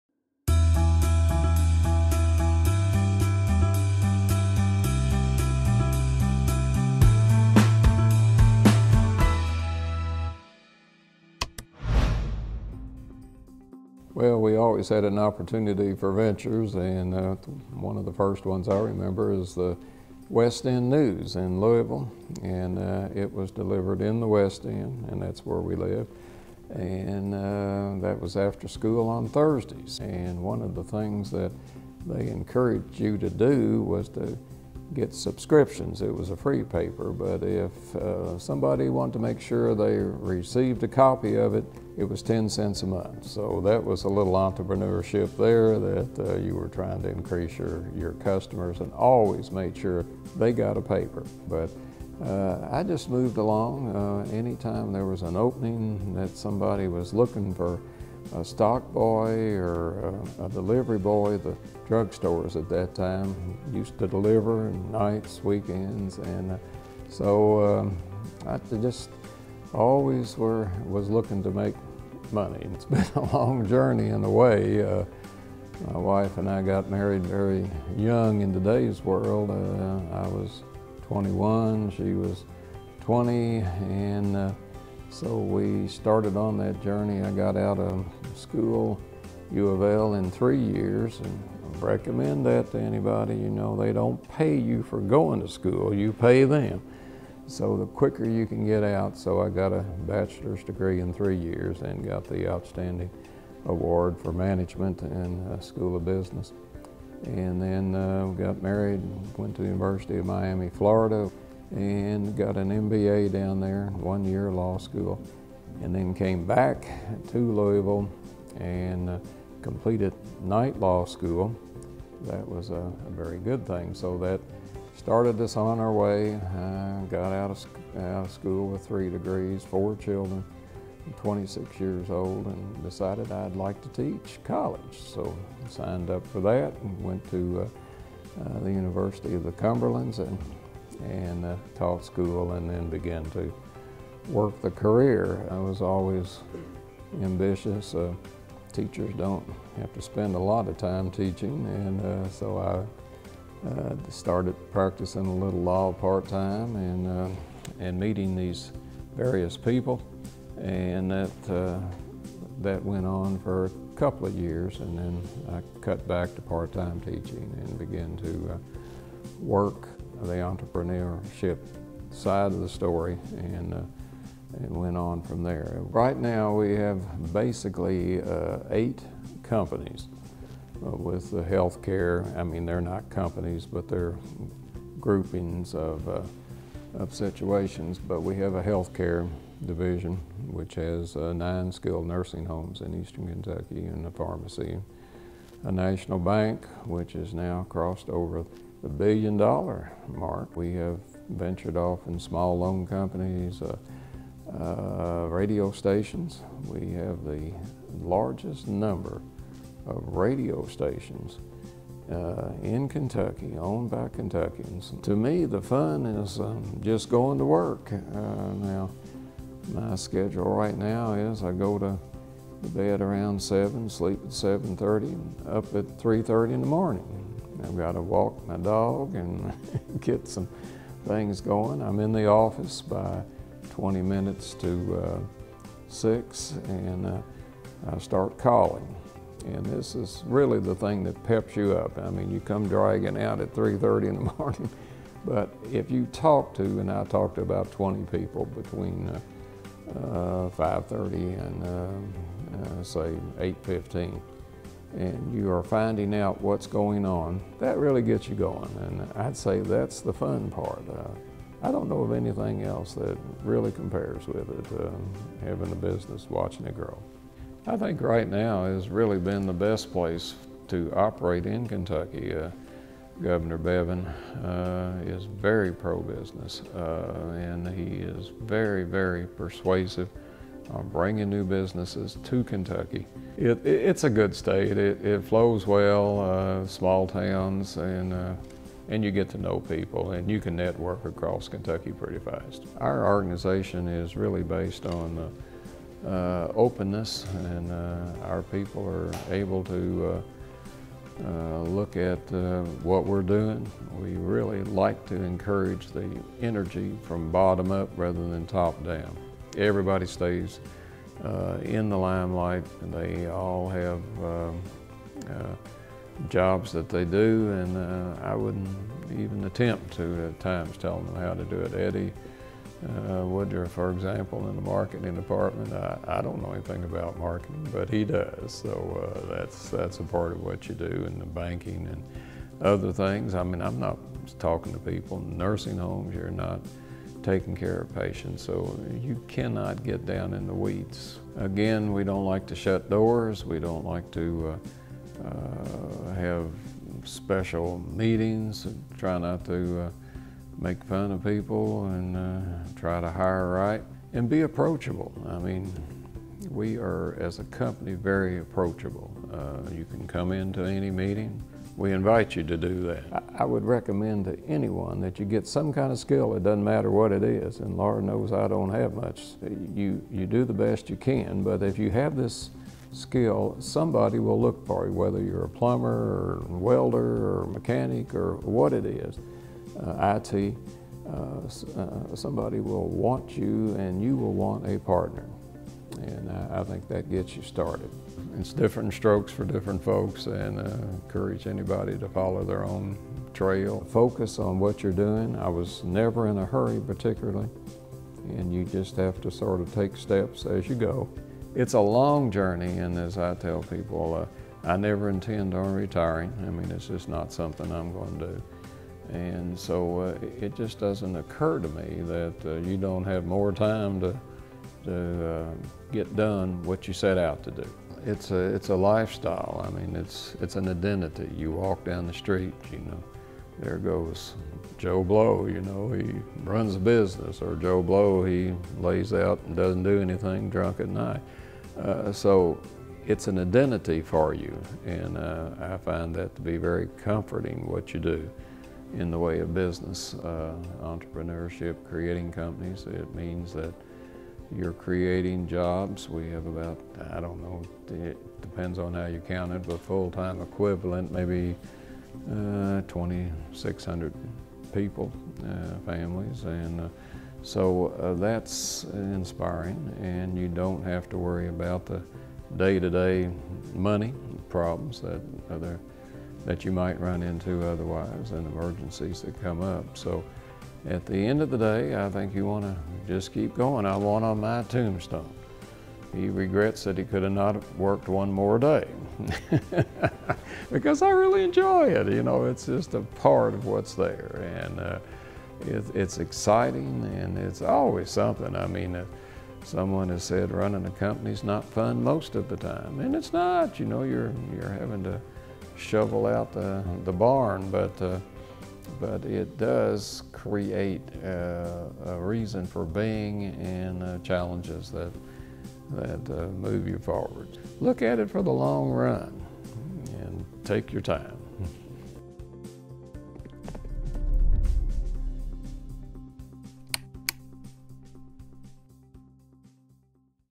Founders Series | Terry Forcht
A 1959 University of Louisville graduate, Terry Forcht has created or acquired nearly 100 businesses during his 40-year career. As founder and Chairman of Forcht Bank, one of the largest bank groups in KY, and Founder, Chairman, and CEO of the Forcht Group of KY, an umbrella organization for 95 separate companies with more than 2,400 employees, Forcht has proven his brilliance through his entrepreneurial endeavors. In 2008, Mr. Forcht donated $3 million to U of L's College of Business, establishing the Forcht Center for Entrepreneurship, where students can expand their exposure to the challenges and opportunities of the business world.